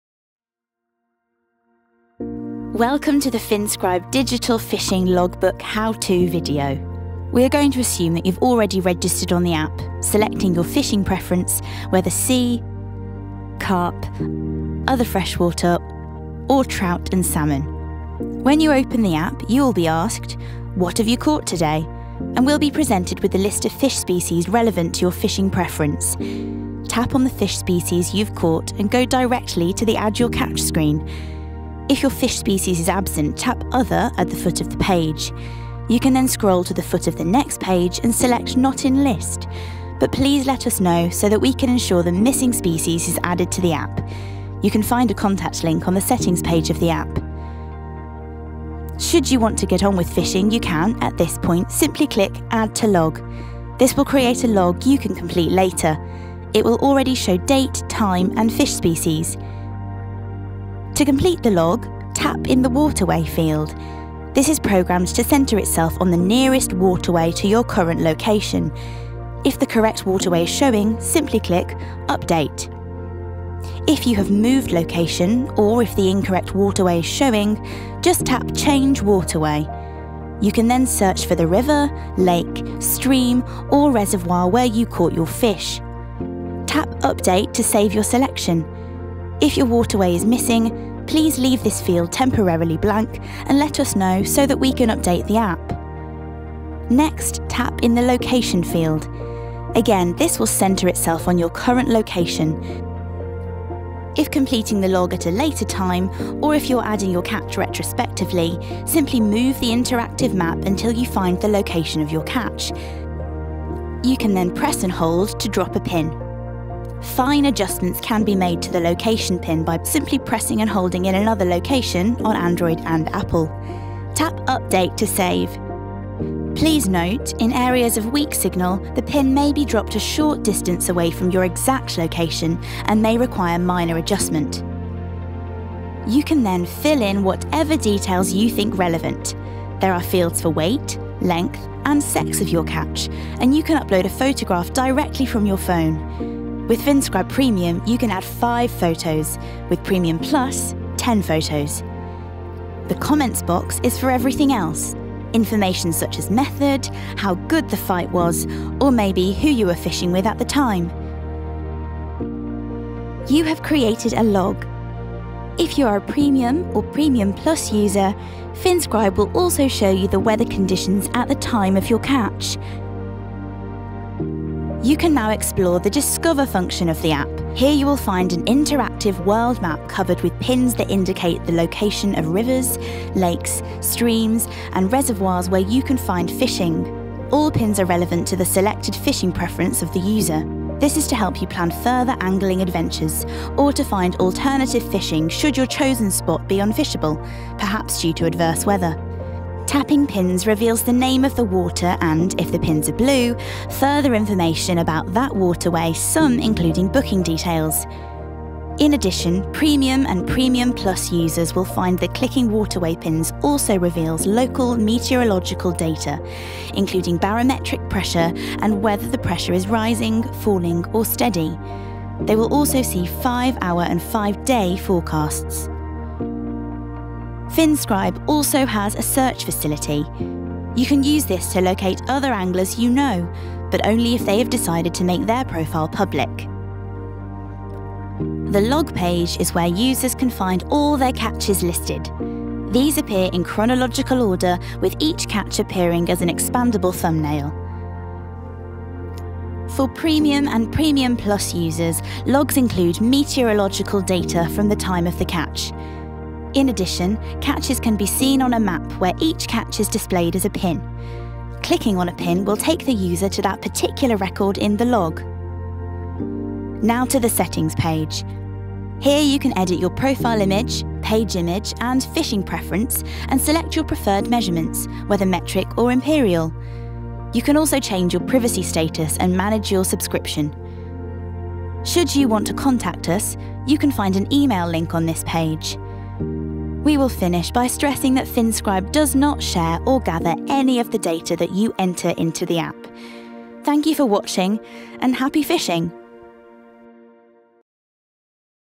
finScribe - How to Use the finScribe app
This video is intended to show users the full scope of functionality of our fishing logbook app, including how to log a fish, how to locate new waters and the benefits of our Premium and Premium+ versions